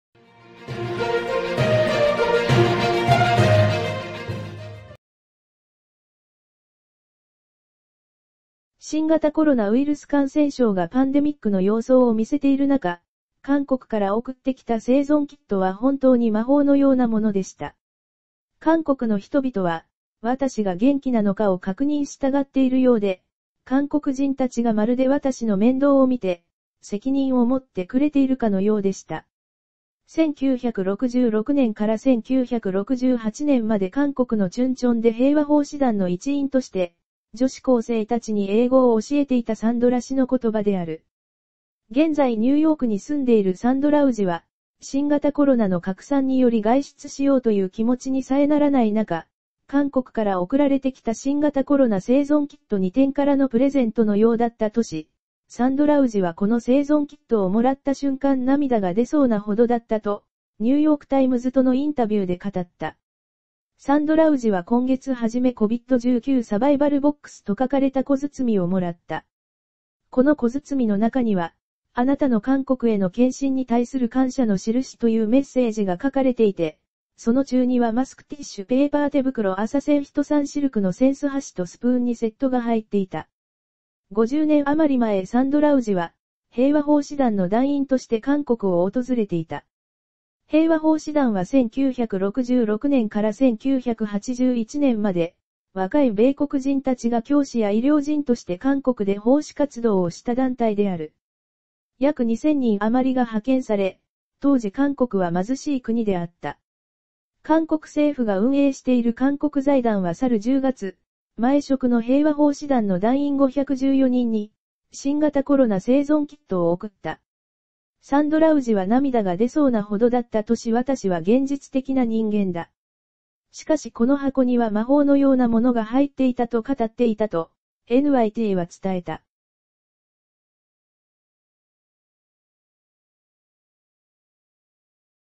韓国から送られてきた“生存キット”に…米“平和奉仕団員”「涙が出そうなほどだ」 (11/22)
韓国から送られてきた“生存キット”に…米“平和奉仕団員”「涙が出そうなほどだ」